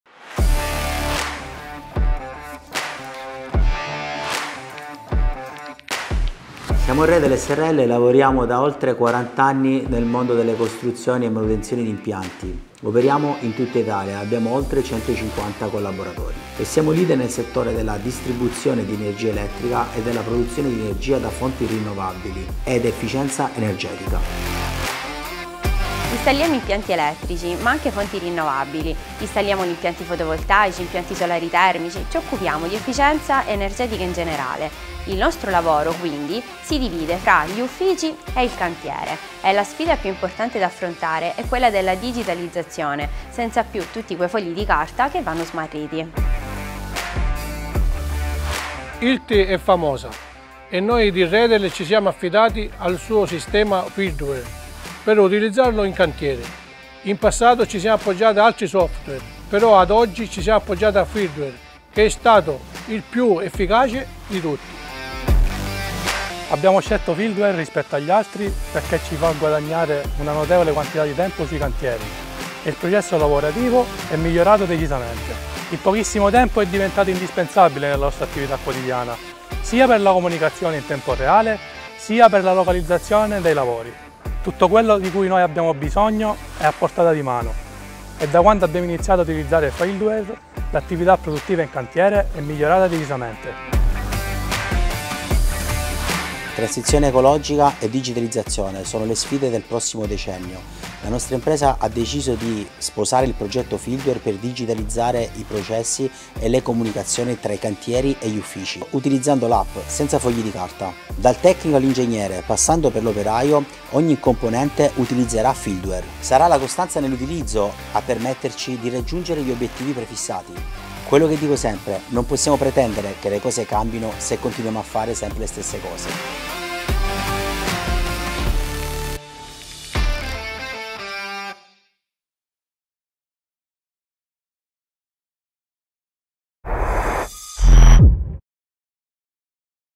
Customer Story - Redel punta sull’innovazione Fieldwire per connettersi con il futuro
👷‍♂️ Redel S.r.l. lavora da oltre 40 anni nel mondo delle costruzioni e manutenzioni di impianti elettrici, e ha scelto Fieldwire per ottimizzare i progetti attivi in tutta Italia.

Con più di 150 collaboratori tra ufficio e campo, l’azienda ha migliorato i processi di lavoro grazie ai vantaggi del software:

⏱ Notevole risparmio di tempo in cantiere

📱 Comunicazione in tempo reale

📍  Localizzazione immediata dei lavori